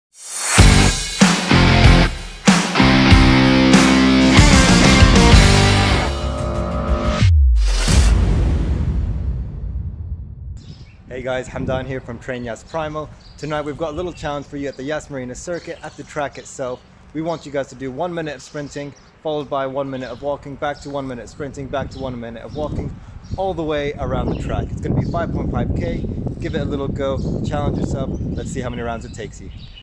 Week 3 - Track Primal Challenge: Speed Play Training 🏃‍♀️🏃‍♂️
🎁Participate and share with us your video or photo using TrainYasPrimal for a chance to enter the draw and win Caterham driving experiences on our track.

الأسبوع الثالث - تحدي اللياقة البدنية على الحلبة: تدريب تغيير سرعة الركض 🏃‍♀️🏃‍♂️
🎁 شاركنا فيديو أو صورة لتمرينك واستخدم الهاشتاج الخاص بالبرنامج للدخول في سحب لفرصة الفوز بتجارب قيادة كاترهام على حلبتنا.